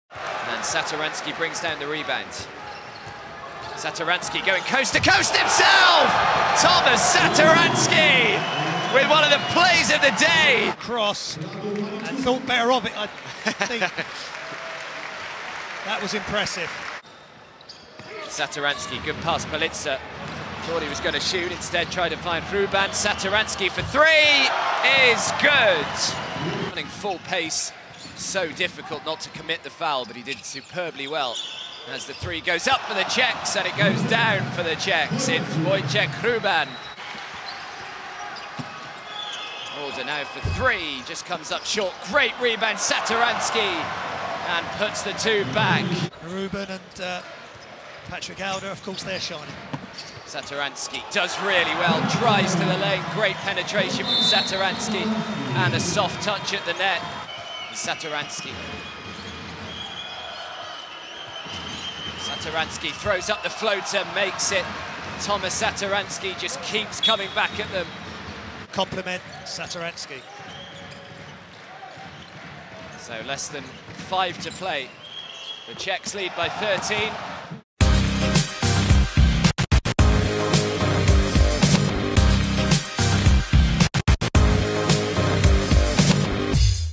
The Tomas Satoransky show against Romania
Tomas Satoransky put on a clinic finishing with 20 points, 13 rebounds and 6 assists, leading 🇨🇿 to an 83-68 win over the hosts Romania